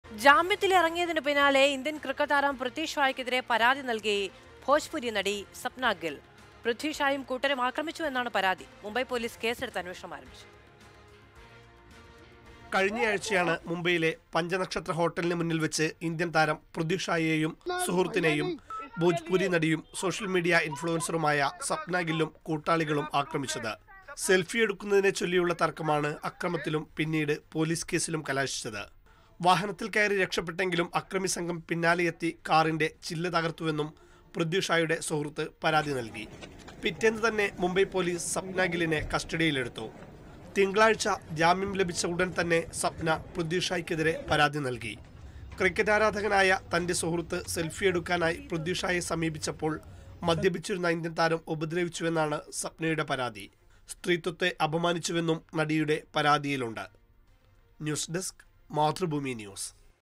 സെല്‍ഫിയെടുക്കാന്‍ വിസമ്മതിച്ചതിനു പിന്നാലെ തര്‍ക്കം; പൃഥ്വി ഷായ്ക്കെതിരെ പരാതിയുമായി നടി സപ്‌ന ഗിൽ
ജാമ്യത്തിലിറങ്ങിയതിന് പിന്നാലെ ഇന്ത്യൻ ക്രിക്കറ്റ് താരം പൃഥ്വി ഷായ്ക്കെതിരെ പരാതി നൽകി ഭോജ്‌പൂരി നടി സപ്‌ന ഗിൽ.

Happy viewing!